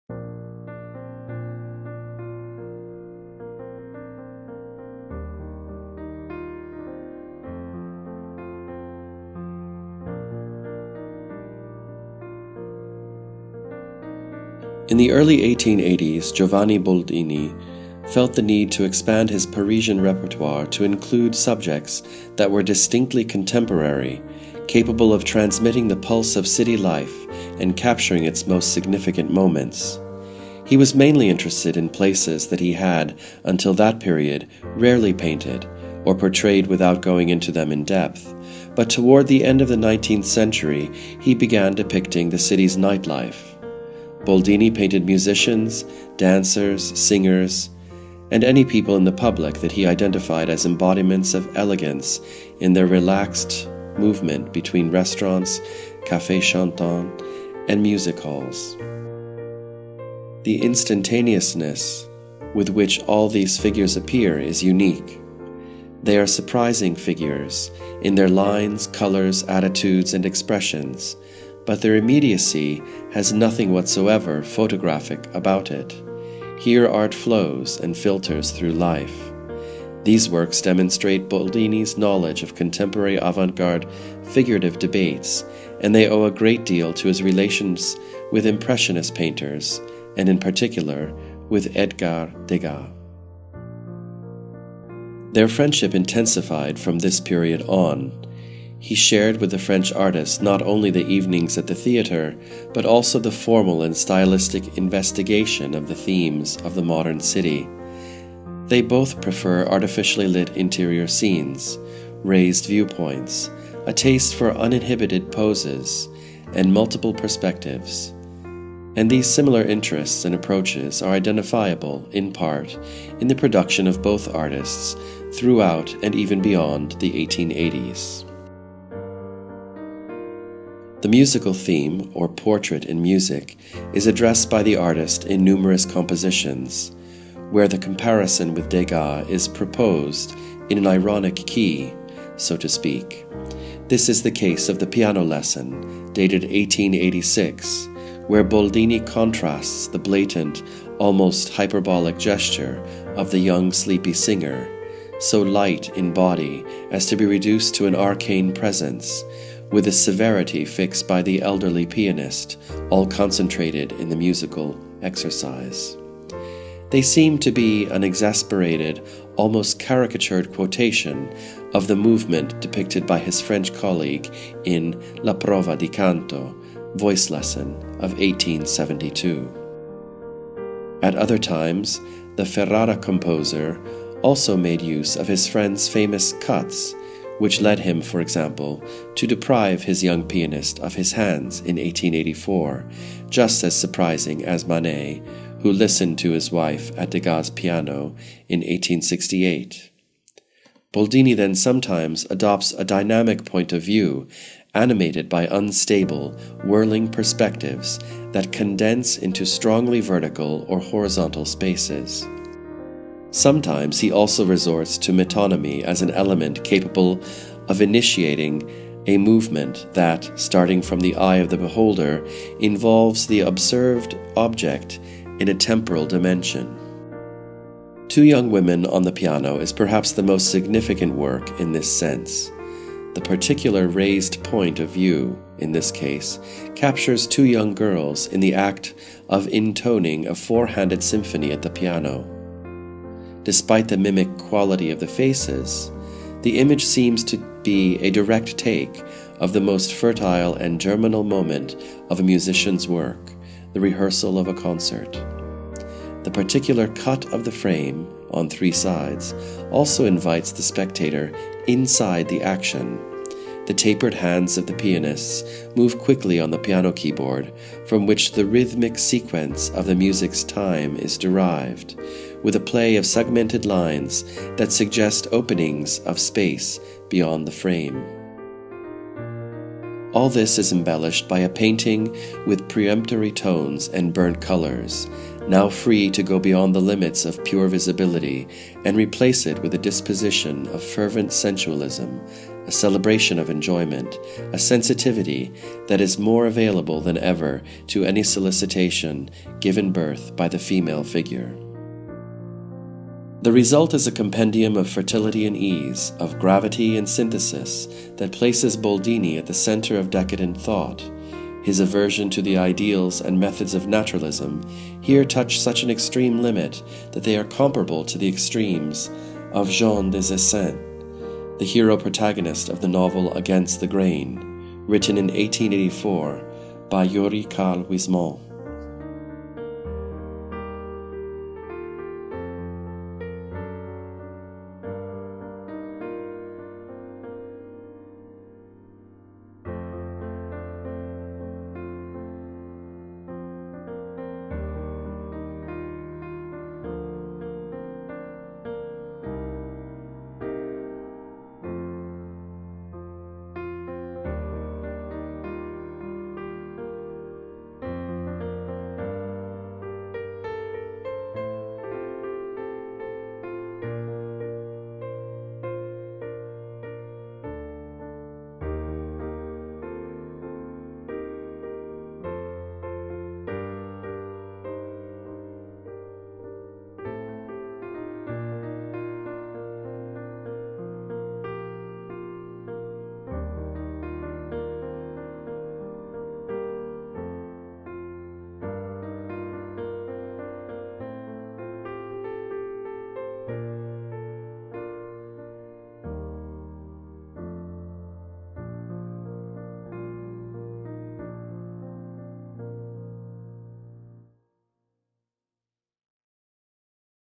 LONDON ART WEEK 2020 - Giovanni Boldini, Two young women on the piano
In this episode that we propose to all of you is the comment on the painting by Giovanni Boldini, "Two young women on the piano" by Stefano Bosi.